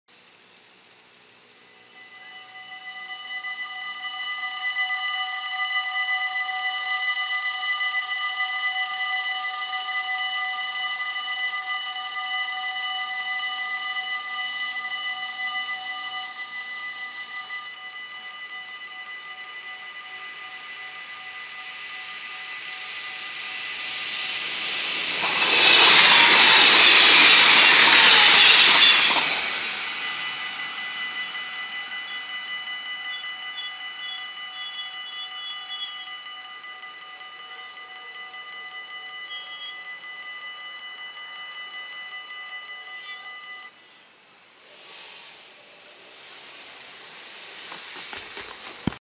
Spoorwegovergang Emmen // Dutch railroad crossing
Locatie: Nijbracht
Traject: Emmen - Zwolle
Soort: AHOB
Rode lichten: 12
Bellen: 7
Bomen: 6
Andreaskruisen: 5
Passeren:
-GTW-E als RE20 Emmen → Zwolle